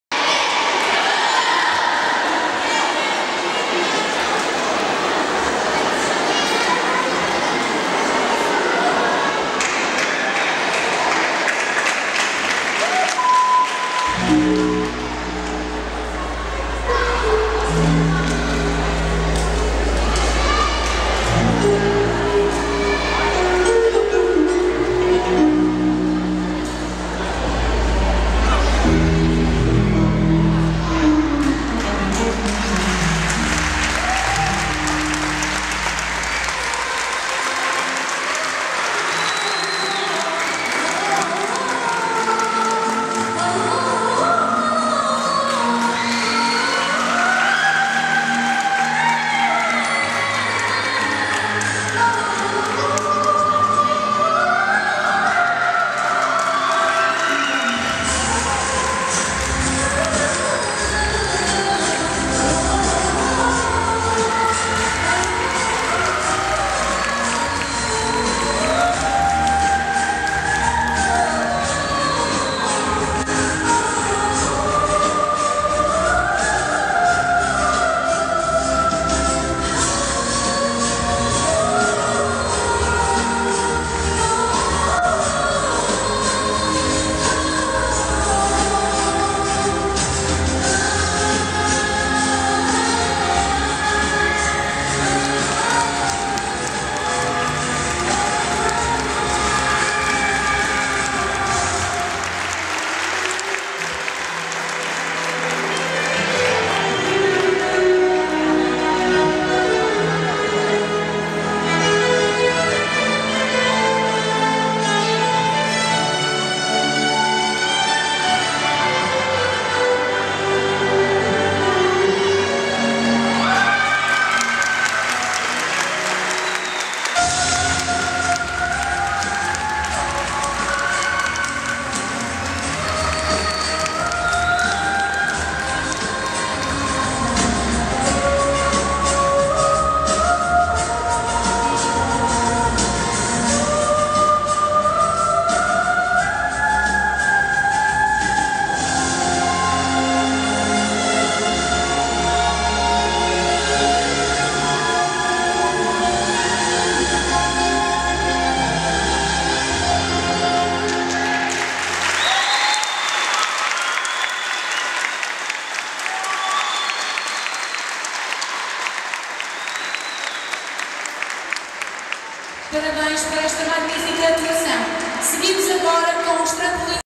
2011_05_Sarau_EDV_Par_Junior GCV
Participação do Ginsásio Clube Vilacondense no Sarau Gymnico do EDV - Viana do Castelo - 27 de Maio 2011